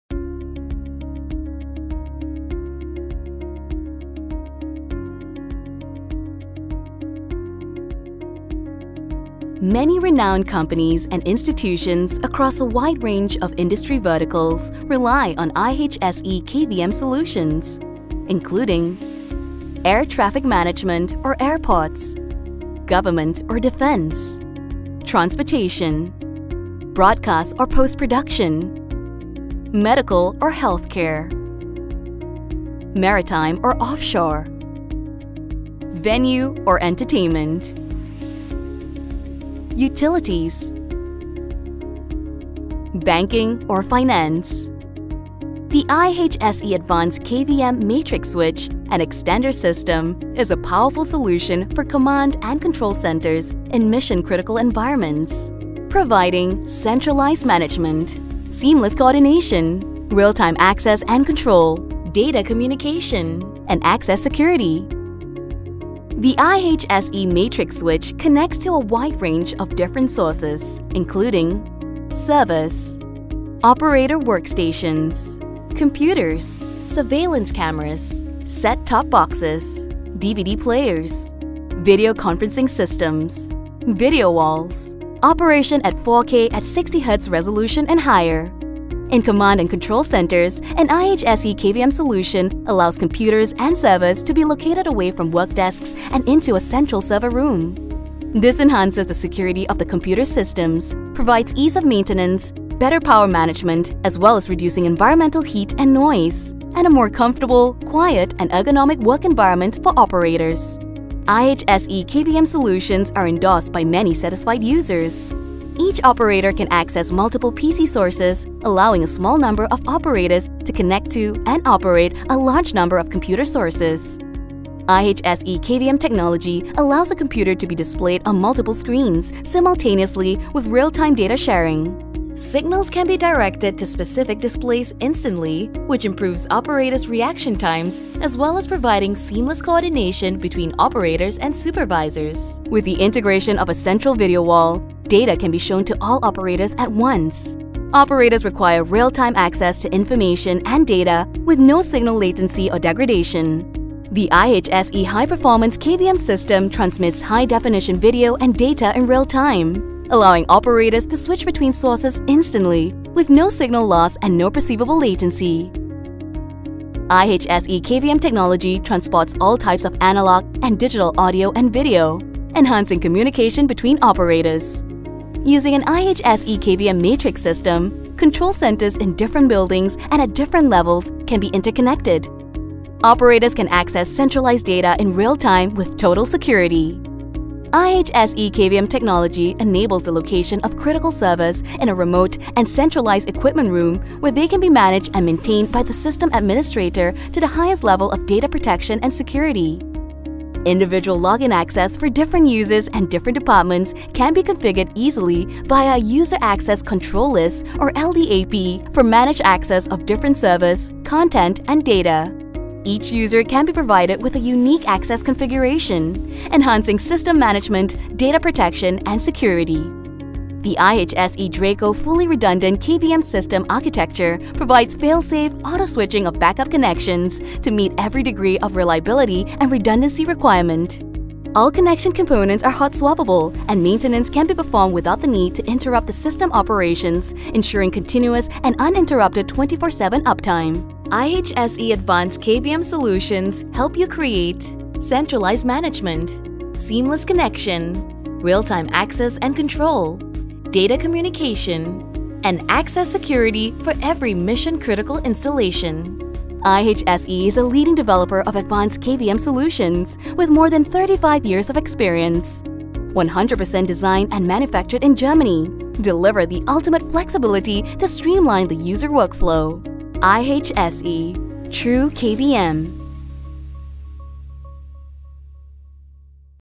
IHSE Advanced KVM Solutions
Discover endless possibilities with IHSE's KVM systems & solutions:

IHSE Matrix switch and extender systems as your powerful solution for command and control centers in mission critical environments, such as:
Air Traffic Management, Security, Transportation, Broadcast, and Healthcare, Maritime, Entertainment, and Finance.

➤ Centralized Management
➤ Seamless Coordination
➤ Real-time Access and Control
➤ Data Communication
➤ Access Security